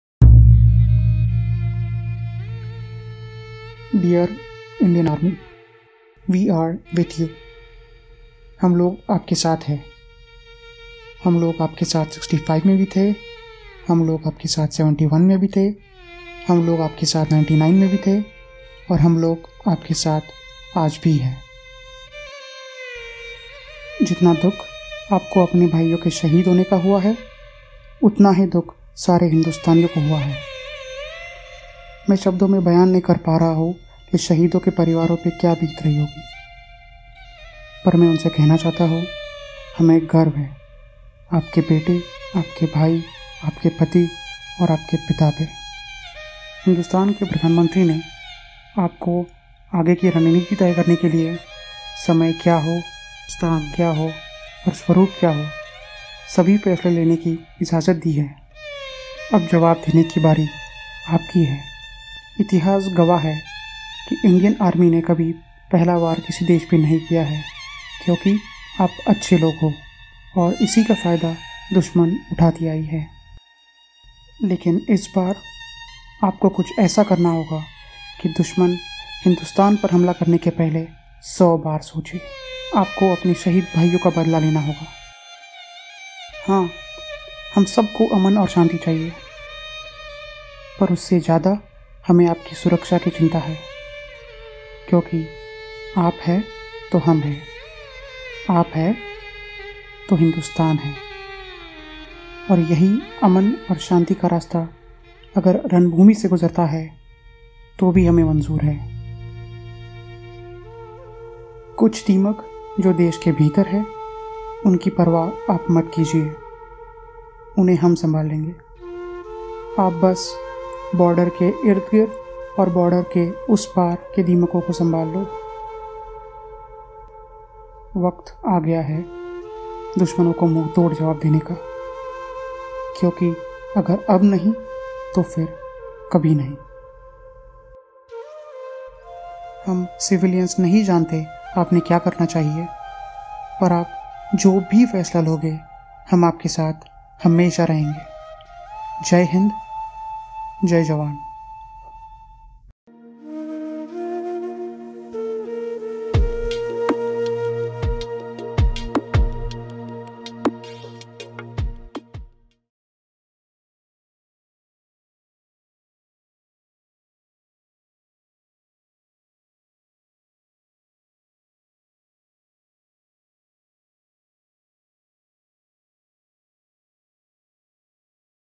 Pulwamaattack - Dear Indian Army I A Letter from Indian Patriot
A message to all Indians to stick together and support the Jawans